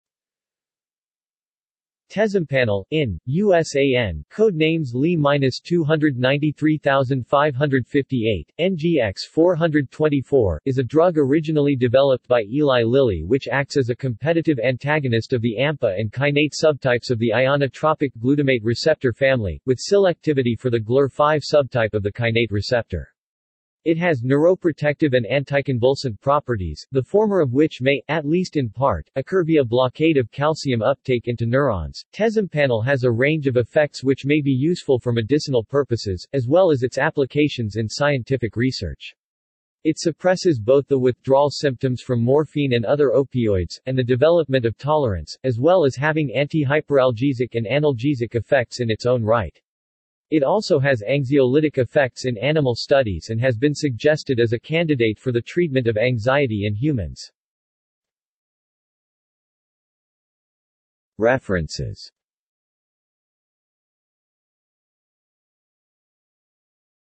Tezampanel | Wikipedia audio article
This is an audio version of the Wikipedia Article:
Tezampanel

SUMMARY
Tezampanel (INN, USAN) (code names LY-293,558, NGX-424) is a drug originally developed by Eli Lilly which acts as a competitive antagonist of the AMPA and kainate subtypes of the ionotropic glutamate receptor family, with selectivity for the GluR5 subtype of the kainate receptor. It has neuroprotective and anticonvulsant properties, the former of which may, at least in part, occur via blockade of calcium uptake into neurons.Tezampanel has a range of effects which may be useful for medicinal purposes, as well as its applications in scientific research. It suppresses both the withdrawal symptoms from morphine and other opioids, and the development of tolerance, as well as having antihyperalgesic and analgesic effects in its own right. It also has anxiolytic effects in animal studies and has been suggested as a candidate for the treatment of anxiety in humans.